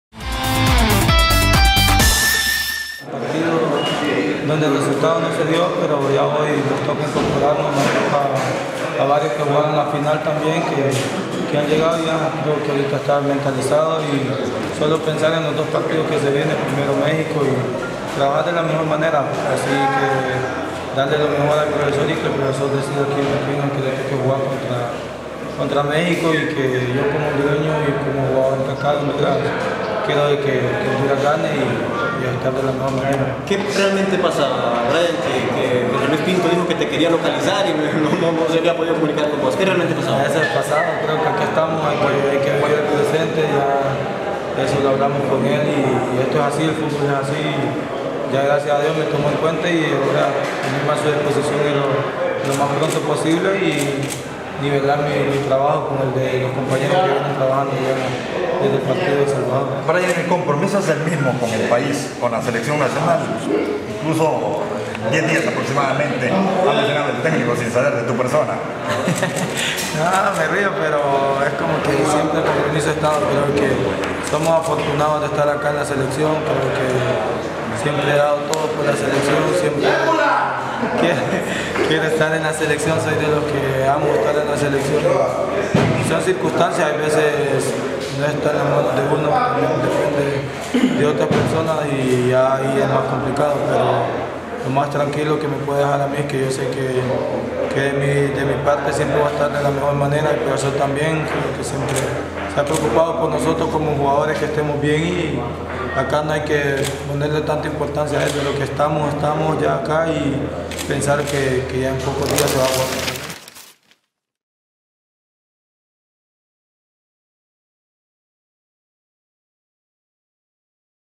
Beckeles aparece y se integra a la Selección de Honduras
El entrenador Jorge Luis Pinto lo daba como desaparecido al defensor.